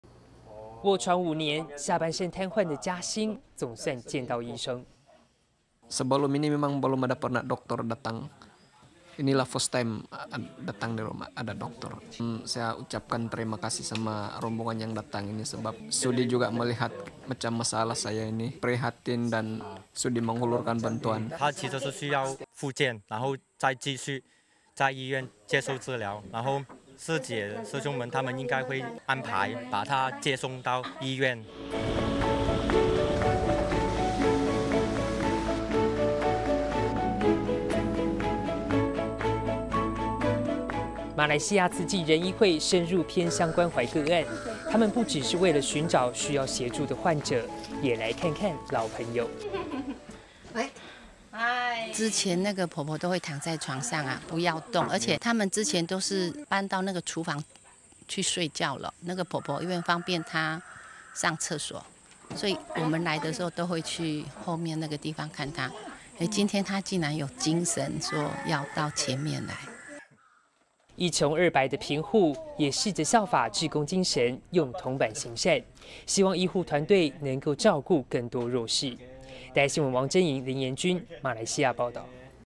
長臥病榻缺關愛 人醫往診視猶親
馬來西亞沙巴的偏遠村落，醫療資源匱乏，當地許多患者，可能一輩子，都無法外出就醫，慈濟人醫會的醫護團隊，前往一個原住民部落，過程需要跋山涉水，為的是守護原民的健康。

臥床五年，下半身癱瘓的「嘉辛」，總算見到醫生。

慈濟照顧戶 嘉辛：「以前沒有醫生要來我家，謝謝你們來幫我看診，很有愛心，對我伸出援手。」

慈濟人醫會醫師 蔡健聰：「他其實是需要復健，然後再繼續在醫院接受治療，師姊師兄們他們應該會安排把他接送到醫院。」

馬來西亞慈濟人醫會深入偏鄉關懷個案，他們不只是為了尋找需要協助的患者，也來看看老朋友。

慈濟志工 謝秀華：「之前那個婆婆都會躺在床上不要動，而且他們之前都是搬到那個廚房去睡覺的，那個婆婆，因為方便她上廁所，所以我們來的時候都會去後面那個地方看她，今天她竟然有精神說要到前面來。」

一窮二白的貧戶，也試著效法志工精神，用銅板行善，希望醫護團隊、能夠照顧更多弱勢。

王楨瑩 林妍君 馬來西亞報導